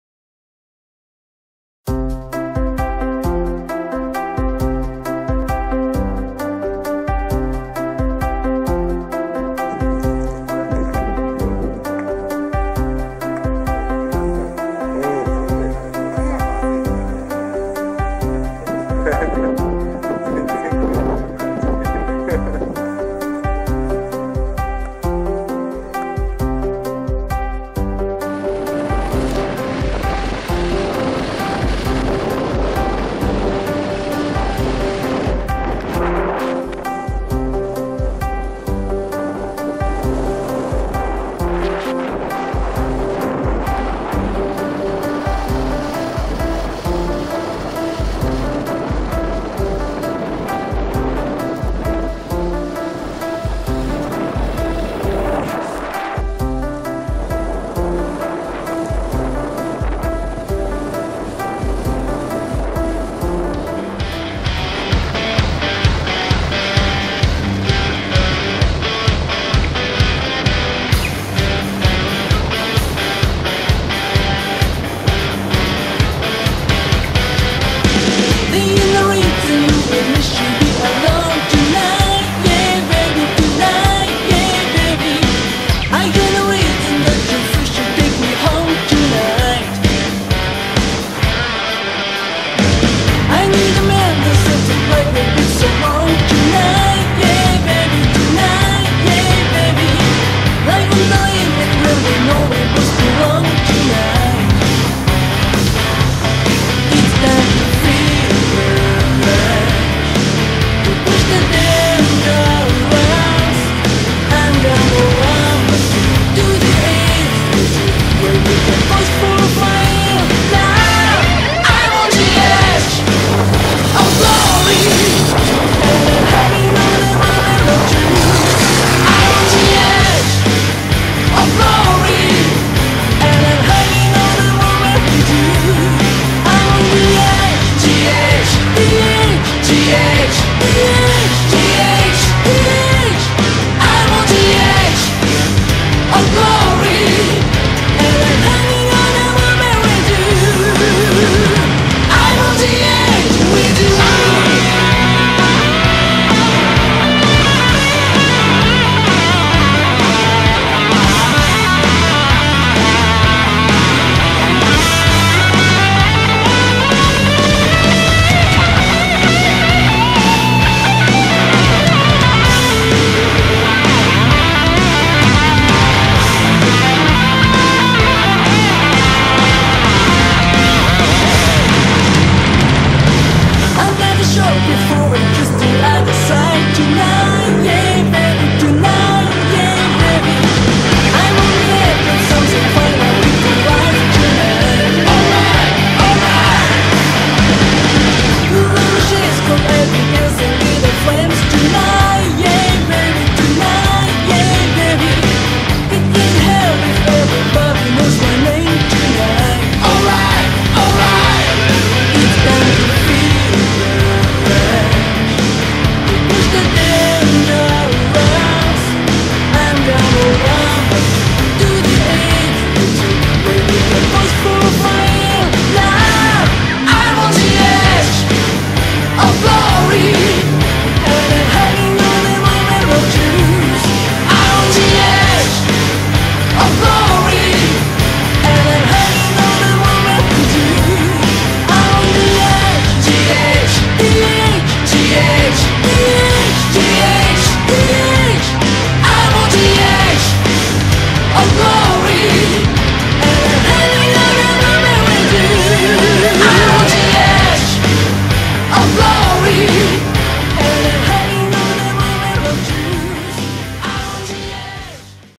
2017猛暑 関西からのライダーさん阿蘇歓迎ツー
関西から九州に来られたタンデムライダーさんを阿蘇までご案内しました。
南阿蘇グリーンロードからスタートで赤牛丼！
産山越えて絶景の中を楽しみました。